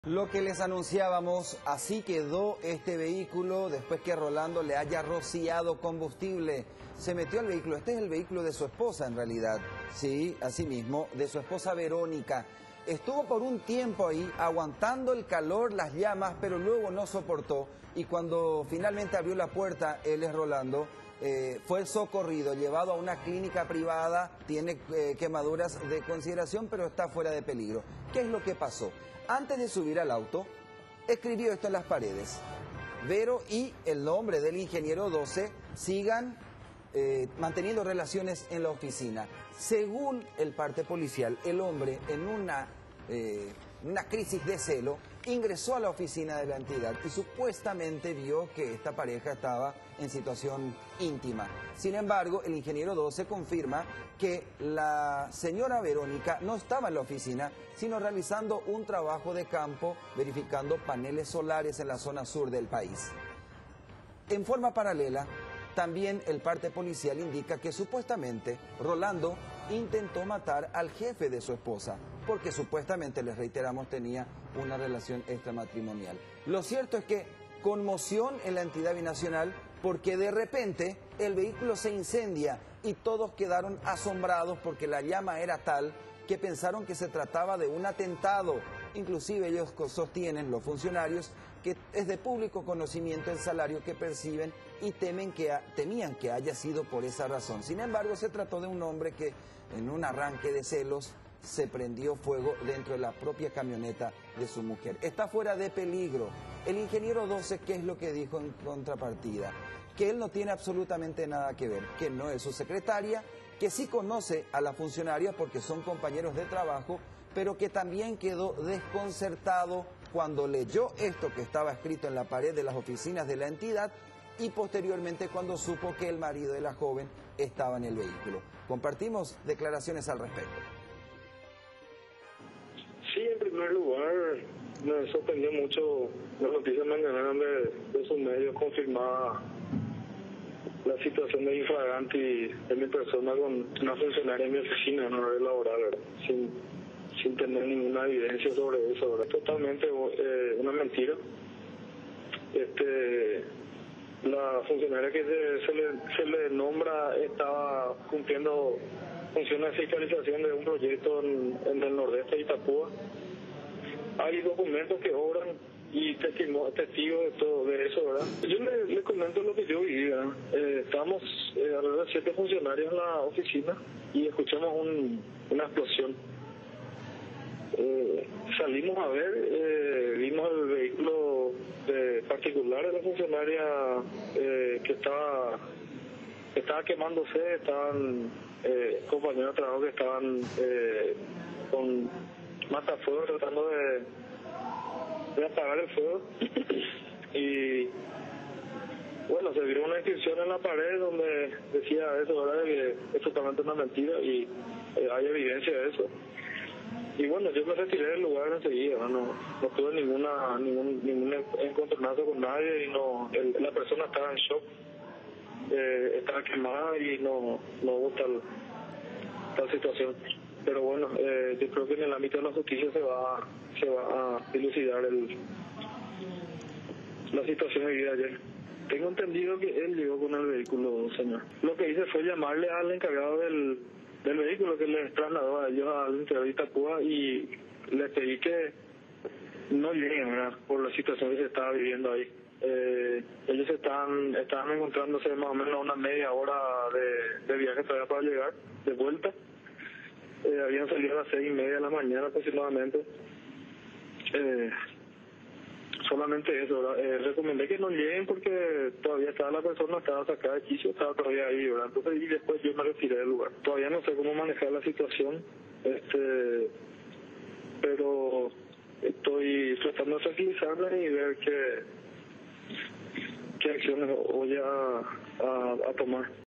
Escándalo en EBY: Uno de los implicados cuentan su verdad 17/03/2016
Uno de los supuestos implicados en el escándalo de celos en la EBY niega tener participación en los hechos.